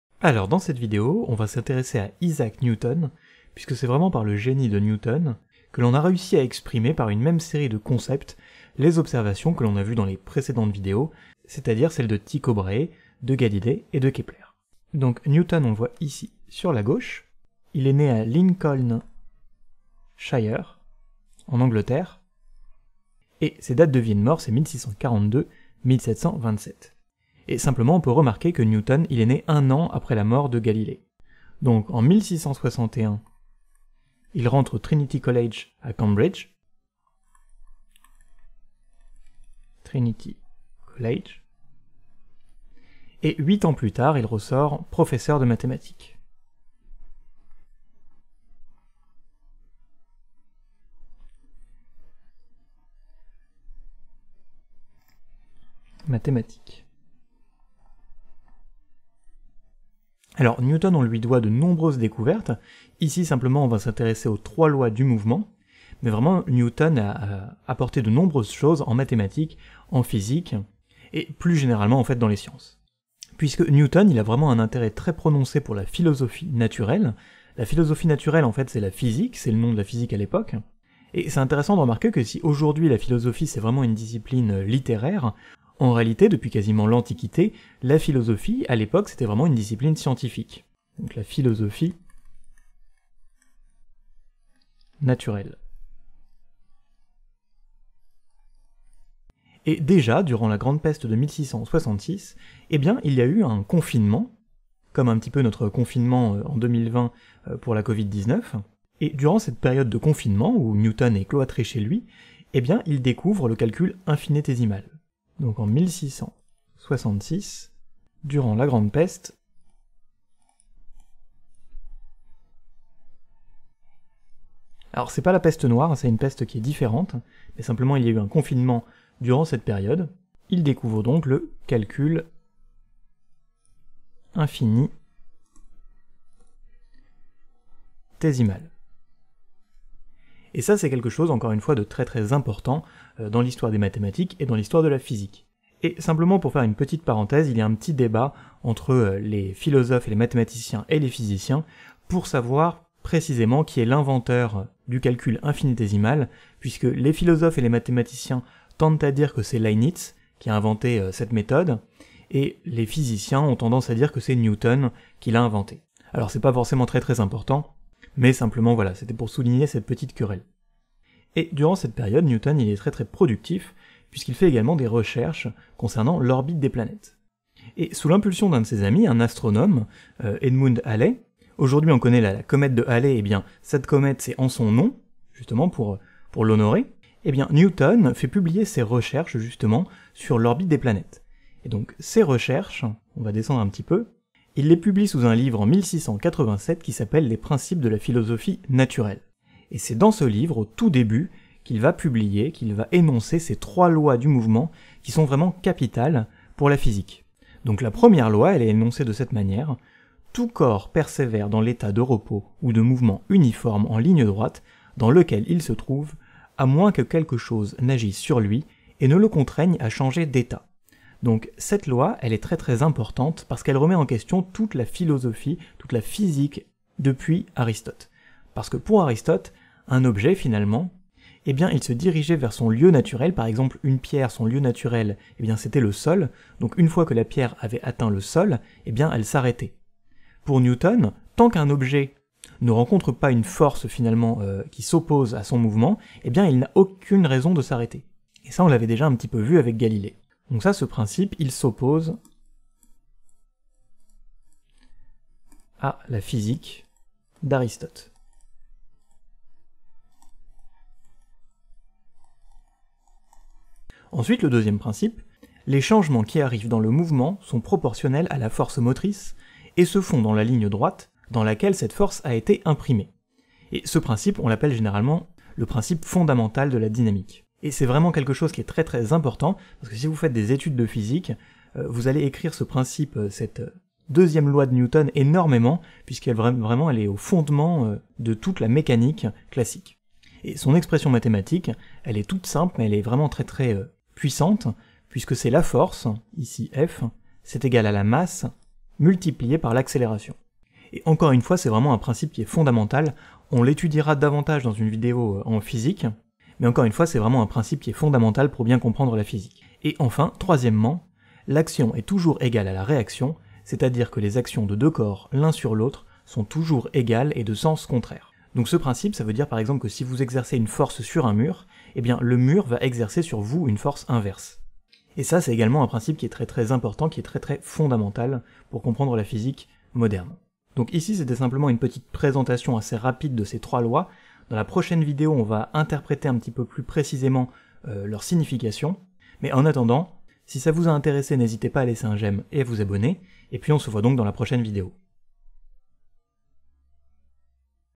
Astronomie - Les trois lois du mouvement de Newton
Retrouvez-moi en live sur Twitch Dans cette vidéo nous allons faire une présentation rapide de la vie de Newton et des trois lois du mouvement de Newton afin de comprendre plus facilement par la suite la gravitation.

Table des matières:
0:00 - Présentation de Newton
3:12 - Les trois lois du mouvement